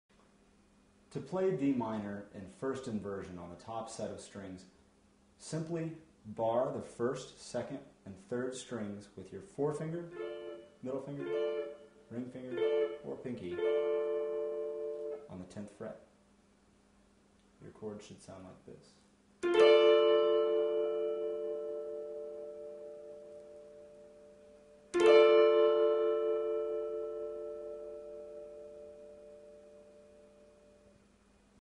1st Inversion of D Minor Guitar Chord on the Top Strings : Guitar Chord Dictionary 21
Watch expert guitarist's finger placement on advanced guitar chords like the 1st inversion of D minor on the top strings of a guitar in this free music video on guitar chords.